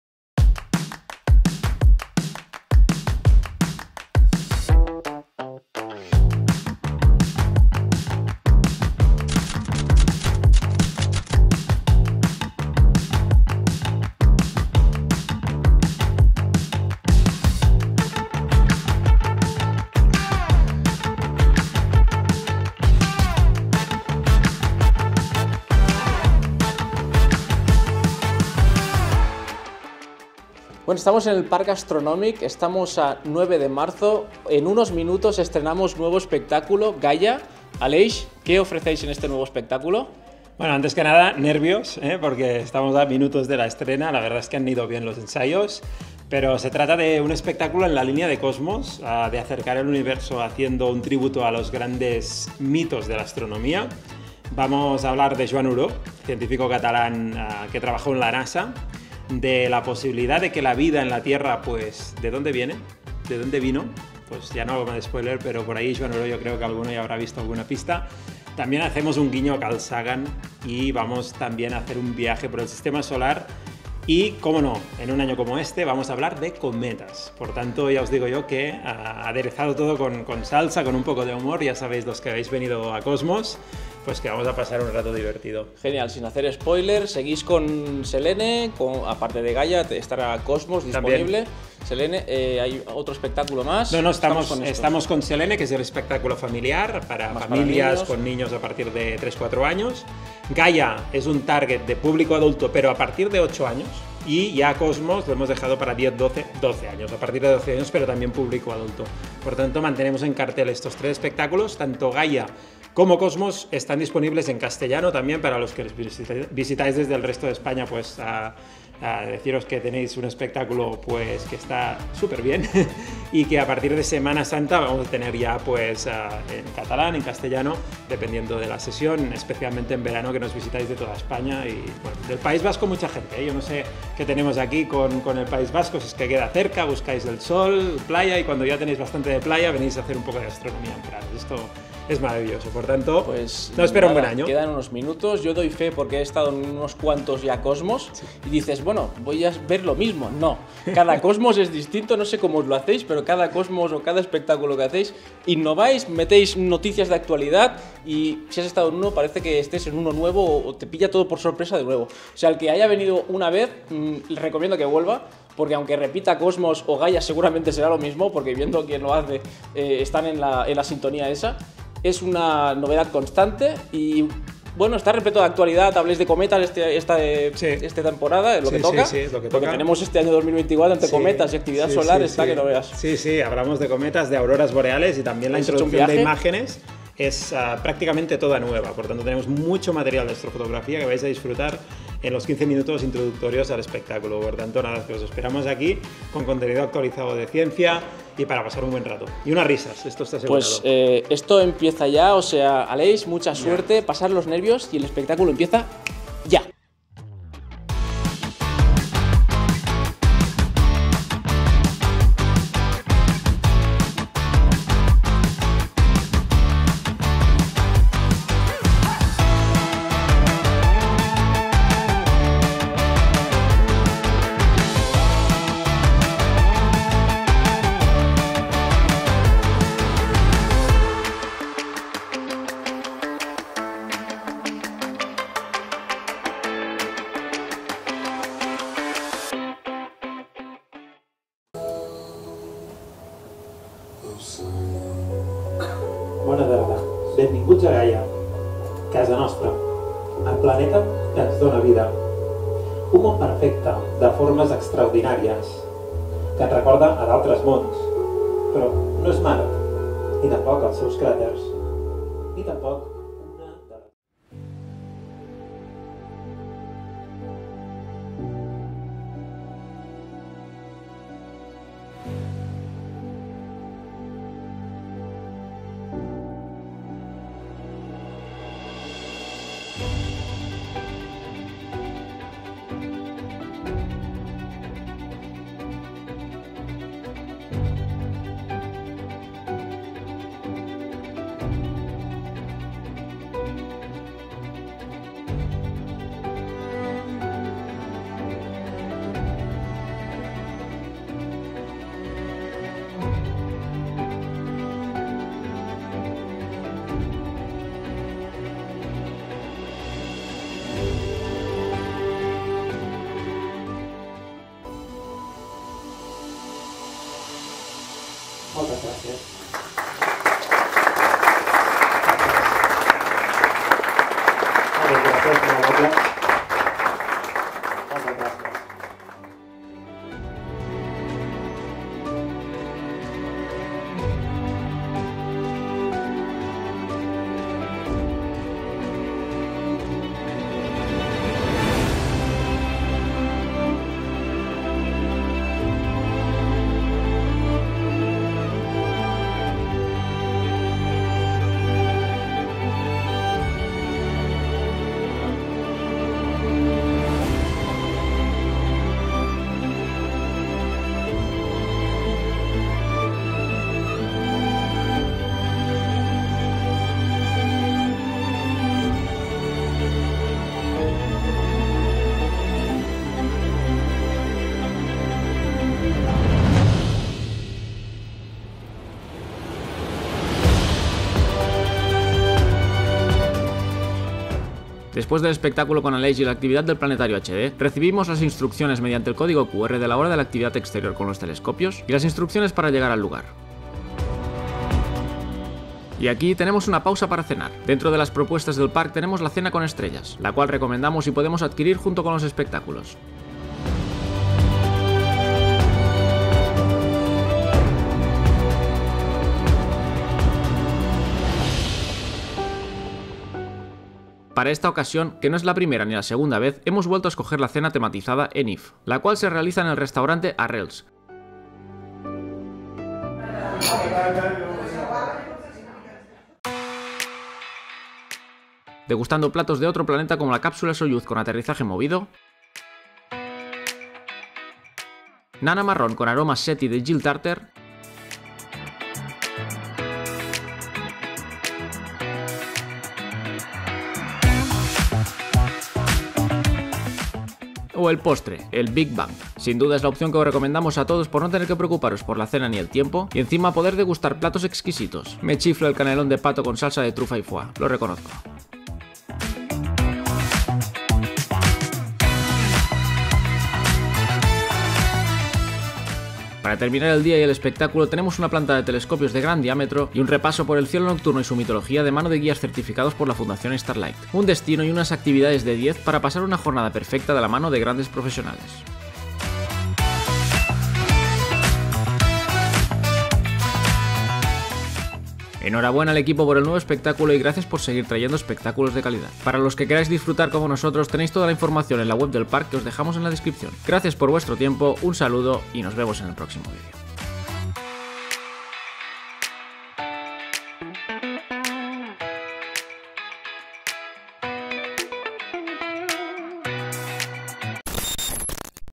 👉GAIA | Nuevo espectáculo del Parc Astronòmic Muntanyes de Prades
Nuestros amigos del Parc Astronòmic Muntanyes de Prades estrenaron nuevo espectáculo el pasado 9 de marzo, GAIA, y como no podía ser de otra manera, estuvimos ahí. En este video os compartimos nuestra experiencia ese día y alguna recomendación.

☕¿Quieres apoyar al canal e invitarme a un café virtual y colaborar al bienestar de animales abandonados?